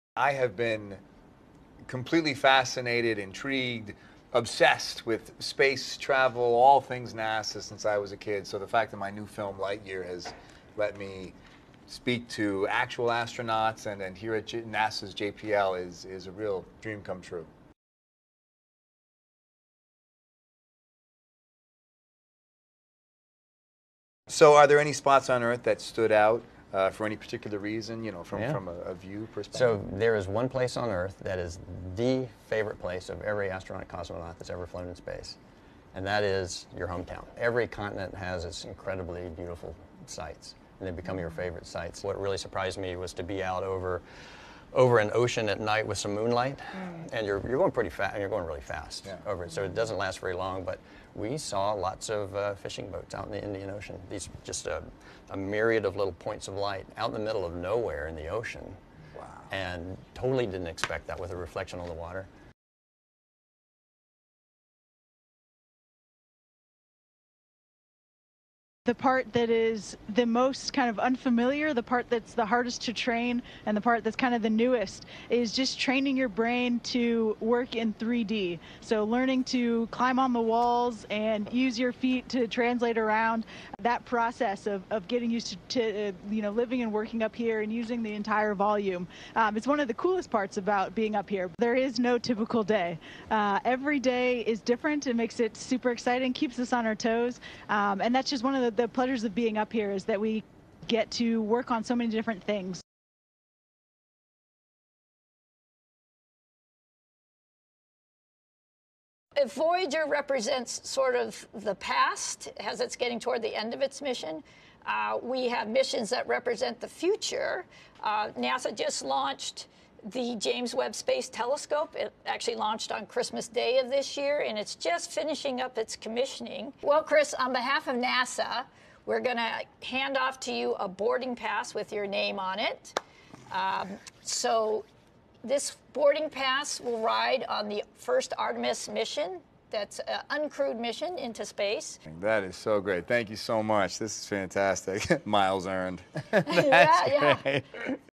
Chris Evans visited NASA JPL and interviewed Astronaut Tom marshburn
nasa jpl As part of the kickoff for Lightyear, chris Evans visited nasa JPL and interviewed astronaut Tom marshburn, who consulted on the movie, as well as members aboard the iss.

During their interview, Chris asked Tom Marshburn about what it’s like to see Earth from space and astronaut Jessica watkins mentioned the hardest part of adjusting to work in zero gravity.

Chris also spoke with Suzy Dodd, project manager for Voyager. She talked about the images that will be released from nasa Webb, which will give us more insight into the mysteries of our universe. Chris ended his interviews with a little souvenir – a NASA Artemis boarding pass that will travel on the uncrewed Artemis I mission to the Moon.